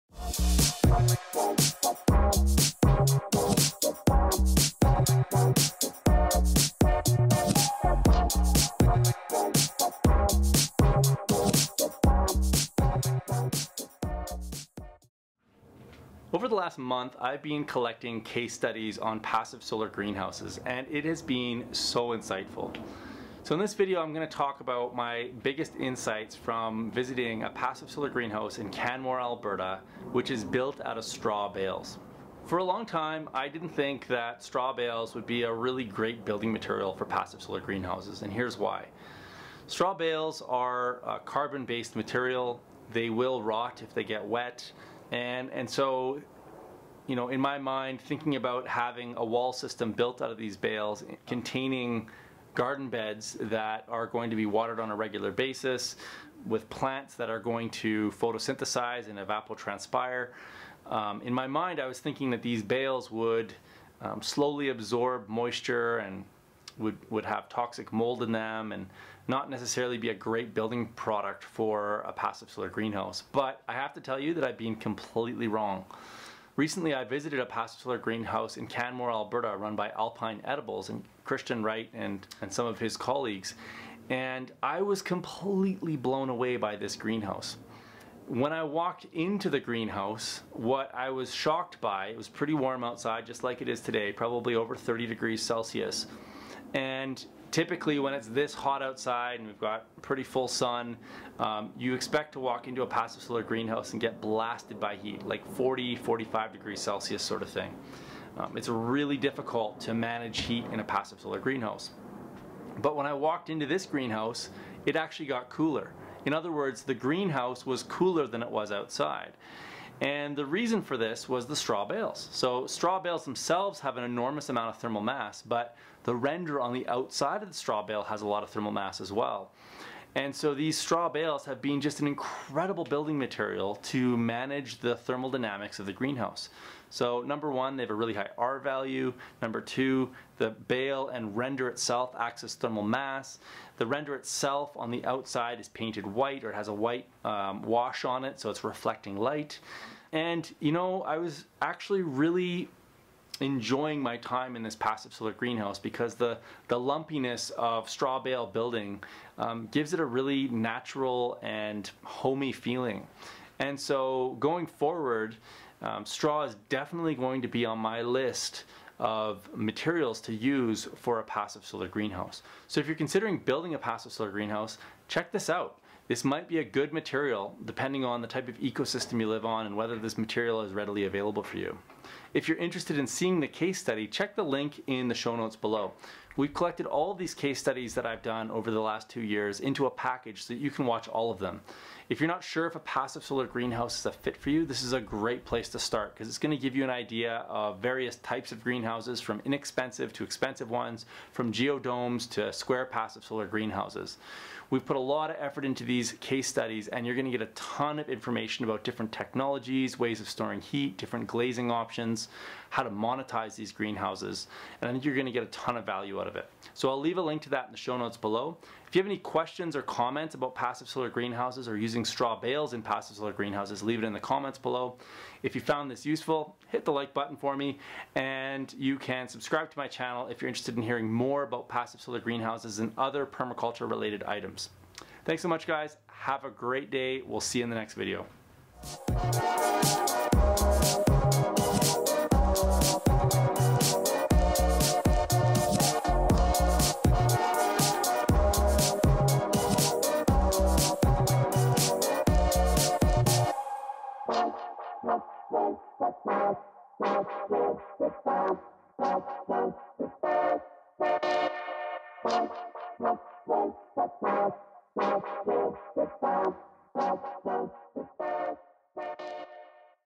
Insights From A Strawbale Greenhouse
I wasn't a fan of using straw bales s a building material for greenhouses, then I saw this one! Watch this video to learn of my insights into straw bale greenhouses and see if you should consider using straw bales in your passive solar greenhouse!